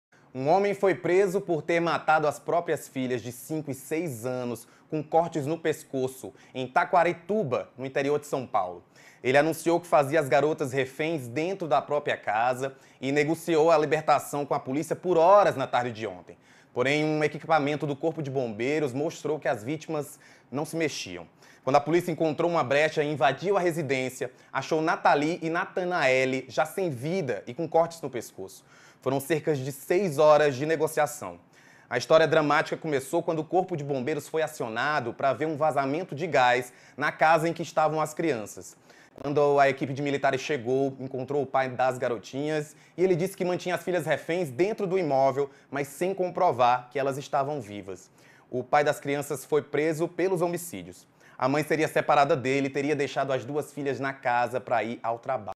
SP: pai mata filhas e por horas disse que elas estavam como reféns | BOLETIM METRÓPOLES
Um homem foi preso por ter matado as próprias filhas, de 5 e 6 anos, com cortes no pescoço, em Taquarituba, no interior de São Paulo (SP).

Ele anunciou que fazia as garotas reféns dentro da própria casa e negociou a libertação com a polícia por horas na tarde de ontem. Porém, um equipamento do Corpo de Bombeiros mostrou que as vítimas não se mexiam.

Leia a matéria completa no portal Metrópoles.